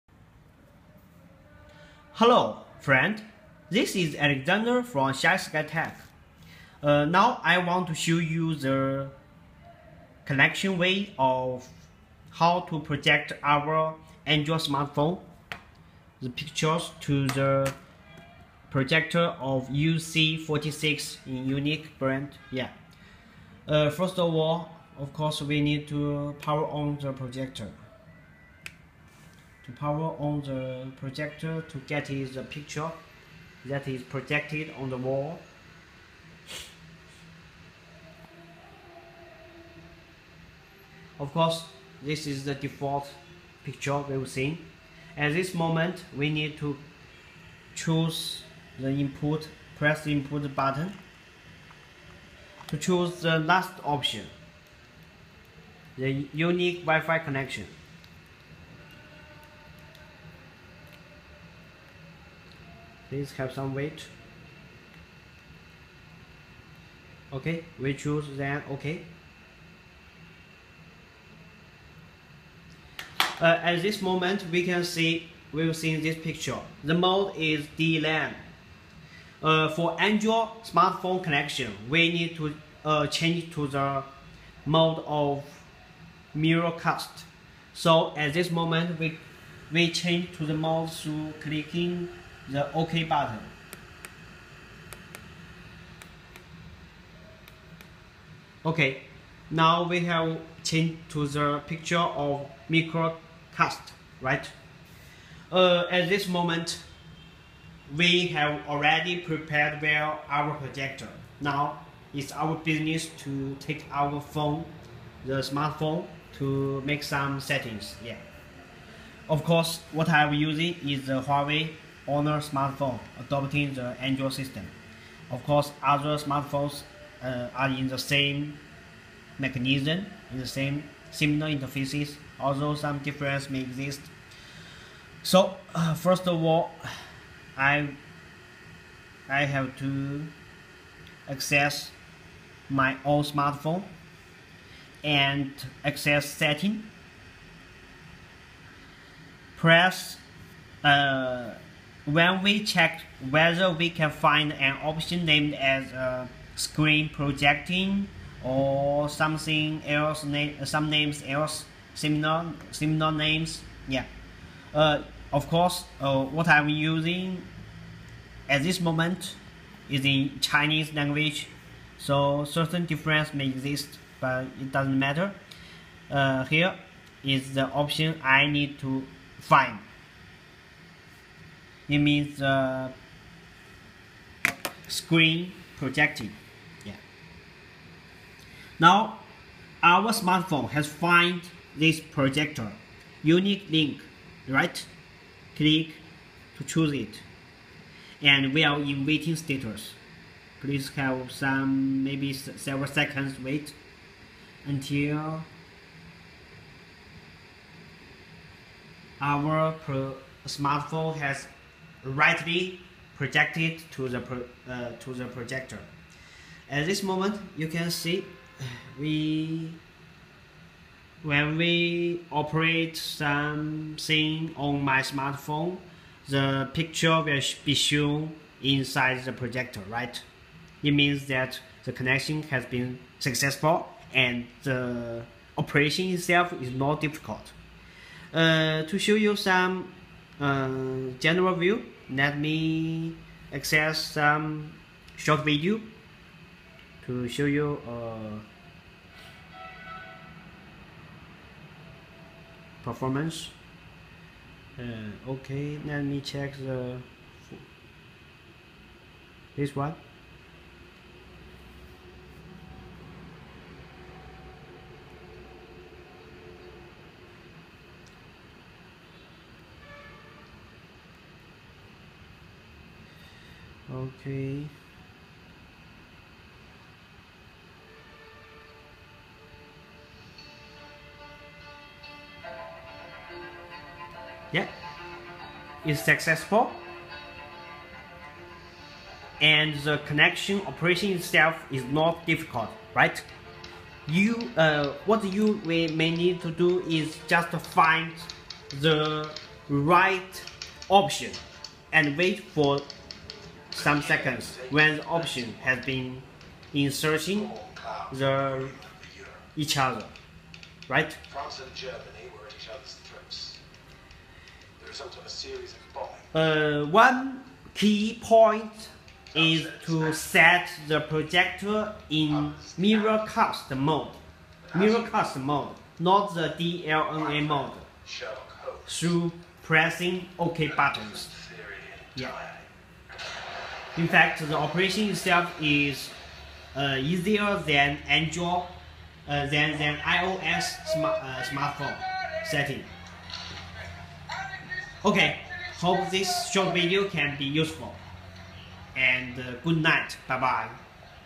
Short video that shows the operation to connect UNIC UC46 with Android smartphone
In this short video, it generally shows the operation to connect UNIC UC46 and an Android smartphone (inside video itself, it's a HUAWEI Honor smartphone). Relevant operational procedure can be concluded as follows:

A. Surely, turn on the projector.
B. Press 'INPUT' button after projector is turned on and choose the last option as source.
C. At this moment, we can see that default mode in the new interface is 'DLAN'. Instead, what we need is 'Miracast'.
How to access 'Miracast'? Press the OK button of remoter or projector itself, to access the 'Miracast' (Maybe you need to press times, not just one time. Perhaps).
D. At this moment, fetch your Android smartphone. Access 'Settings' and choose option of 'screen mirror' (for different mobile phones, the name of this option could be different; but they are in similar equivalent names)
E. Press this right option, then the icon of our UC46 projector will be shown. Press to choose this UC46 projector.
F. Wait for seconds, until the connection is completed. If so, it means that all interface of this smartphone will be timely projected through the projector UC46.
G. Sometimes relevant connection may be not very smooth. If so, just repeat the operations shown on above. Then I believe that you will be successful!

Honestly speaking, it's easier to operate to connect Android smartphone than to connect iPhone or iPad. My personal feeling.

So you can take this demo video as reference. Hope it could be helpful!

Shysky Tech